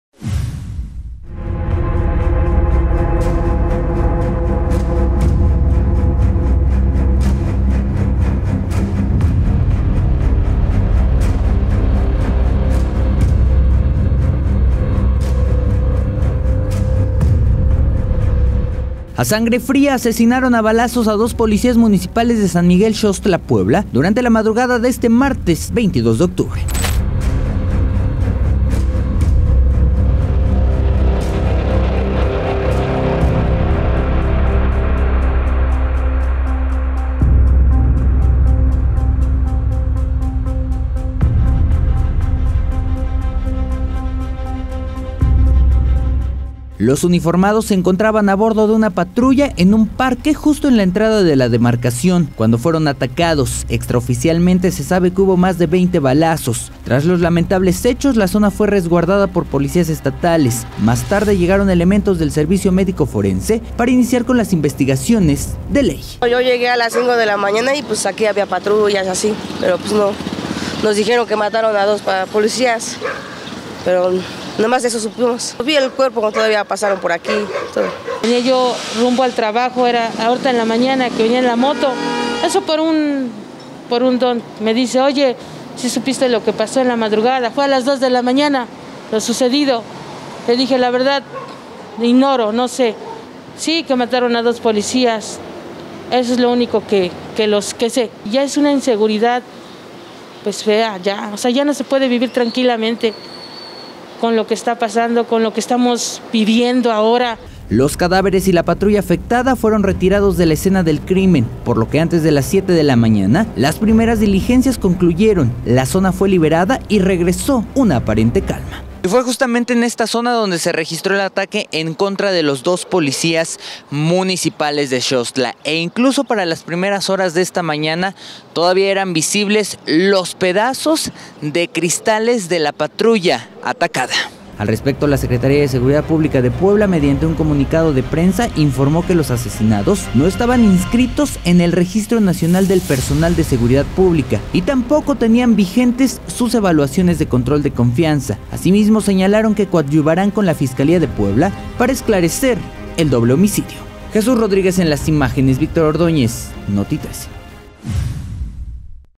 Canal13Puebla 🚔 ¡A sangre fría, fueron asesinados dos policías municipales del municipio de Xoxtla.
Noti13DeLaTarde 🚔 ¡A sangre fría, fueron asesinados dos policías municipales!

🚨 Los uniformados se encontraban a bordo de una patrulla, en el municipio de Xoxtla en Puebla, cuando fueron acribillados a balazos.

🔎 Cabe destacar que los policías asesinados, no contaban con evaluación de control de confianza vigente.